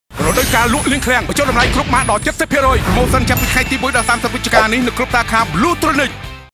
Bluetronics Big Promotion on November
កម្មវិធីប្រូម៉ូសិនពិសេសសម្រាប់រដូវកាលលក់លាងឃ្លាំង ដែលមានការបញ្ចុះតម្លៃរហូតដល់ 70% ។

សូមកុំភ្លេច Bluetronics ផ្ដល់ទំនុកចិត្ត និងសេវាកម្មល្អជូនអតិថិជន!